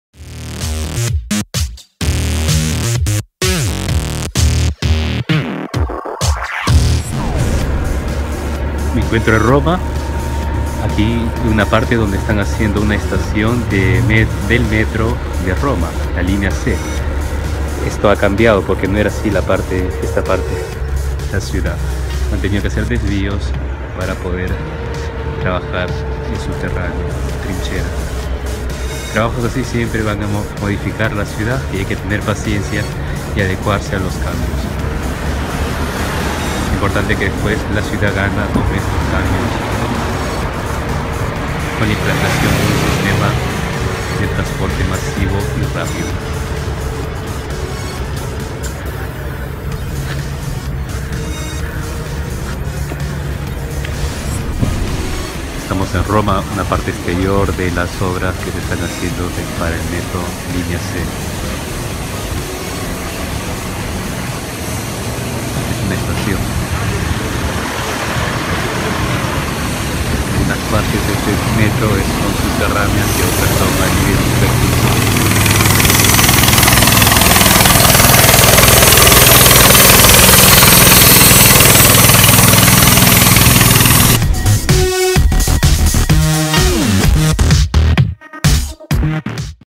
Metro C Roma (2) (Trabajos)
Breve visión de los trabajos que se vienen haciendo actualmente para la Línea C del Metro de Roma